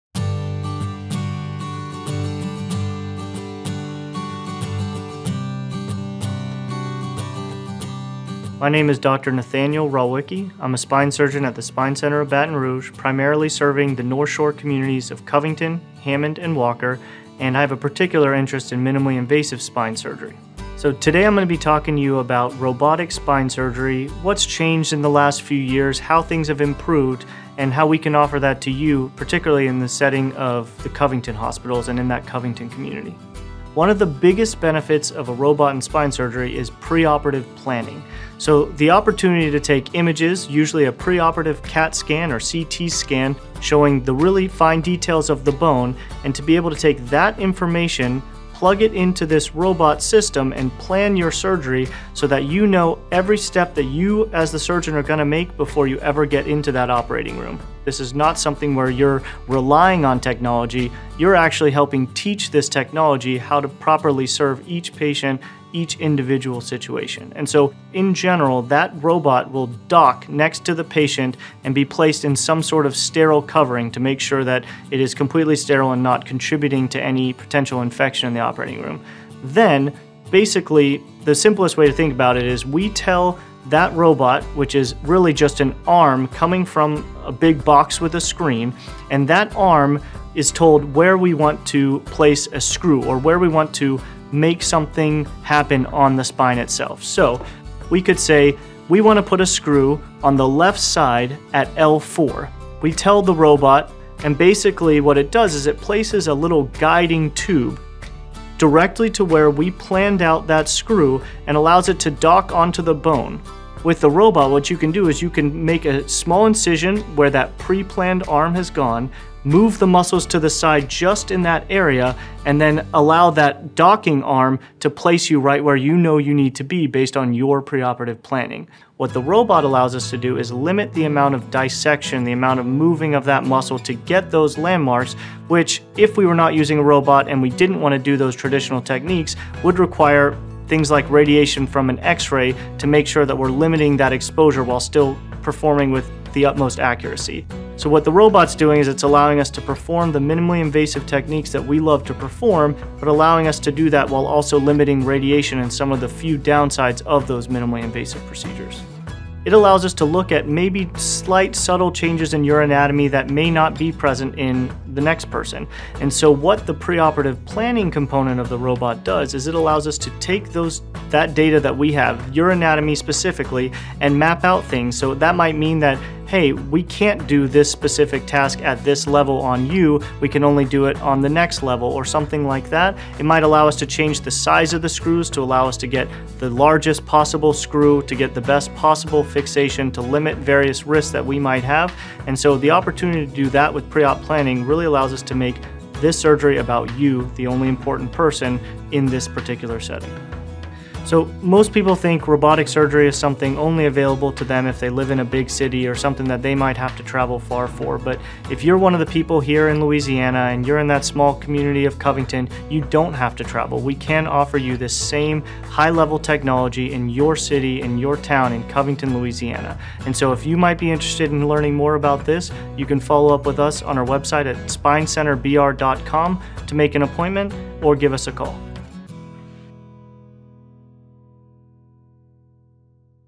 Robotic Spine Surgery Through New Advancements in Technology: The Spine Center of Baton Rouge
The Spine Center of Louisiana

Robotic spine surgery, also known as robotic-assisted spine surgery, is a minimally invasive surgical technique that utilizes robotic systems to assist the surgeon during the procedure. It combines the precision and accuracy of robotic technology with the expertise and decision-making of the surgeon.

During robotic spine surgery, the surgeon uses a robotic system that consists of a robotic arm, specialized instruments, and a 3D imaging system. The surgeon controls the robotic arm, which guides the instruments to perform the surgery with enhanced precision and control.

Potential Benefits:

1. Increased accuracy: Robotic systems provide real-time feedback and guidance to the surgeon, allowing for more precise placement of implants, screws, or instruments. This can result in improved surgical outcomes and reduced risk of complications.

2. Minimally invasive approach: Robotic spine surgery is typically performed using smaller incisions, which can lead to less tissue damage, reduced blood loss, and faster recovery times compared to traditional open surgery.

3. Customized treatment: Robotic systems use preoperative planning and 3D imaging to create a personalized surgical plan for each patient. This allows for a tailored approach that takes into account the patient's unique anatomy and condition.

4. Reduced radiation exposure: Robotic systems often incorporate advanced imaging technology, such as pre- or intra-operative CT scans or fluoroscopy, which can reduce the need for repeated X-rays during surgery. This helps minimize radiation exposure for both the patient and the surgical team.

It's important to note that while robotic spine surgery offers several advantages, it may not be suitable for all patients or all types of spinal conditions. The decision to undergo robotic spine surgery should be made after a thorough evaluation and discussion with a qualified spine surgeon who can determine the most appropriate treatment approach for each individual case.

Call 833-SPINEBR